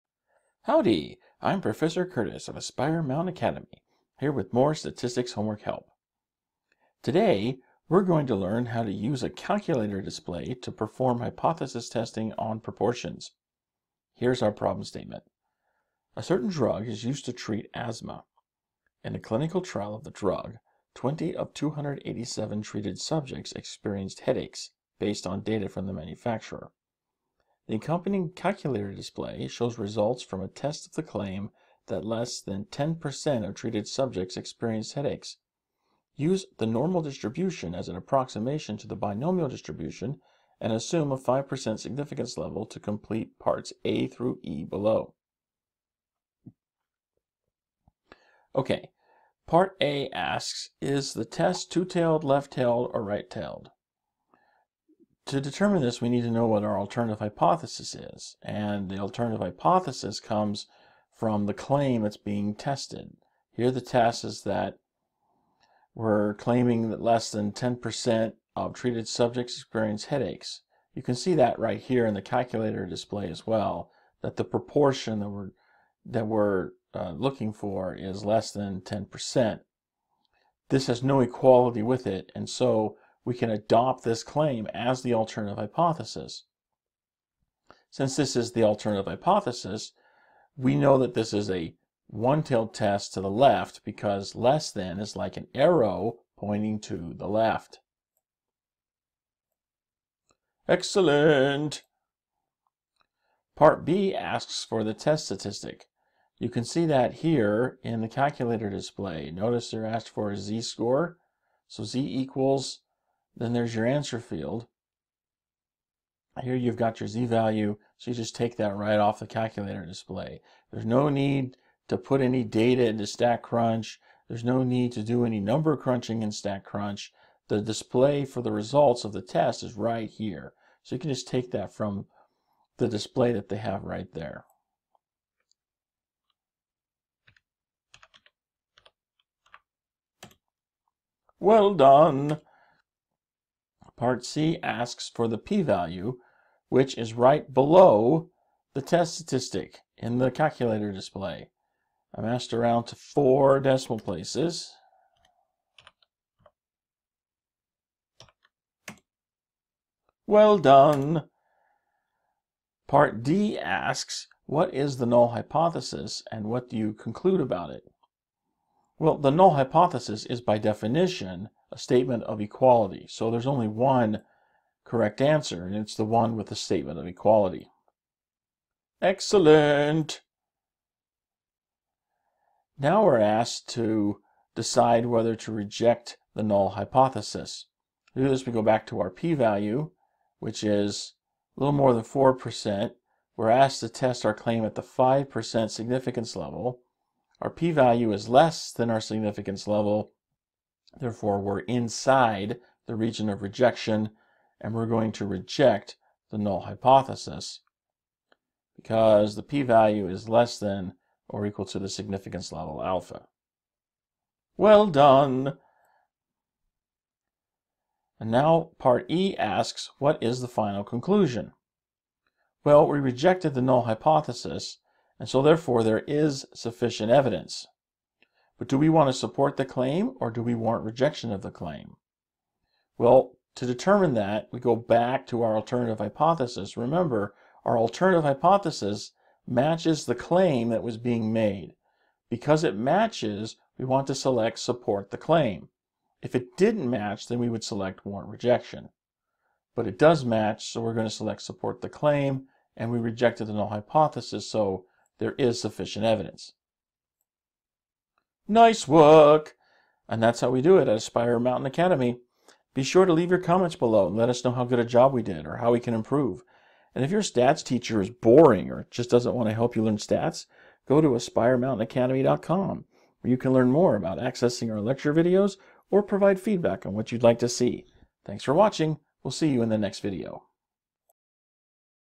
Using a calculator display to perform hypothesis testing on proportions
In this video, Professor Curtis demonstrates how to use a calculator display to perform hypothesis testing on proportions (MyStatLab ID#8.2.5 ).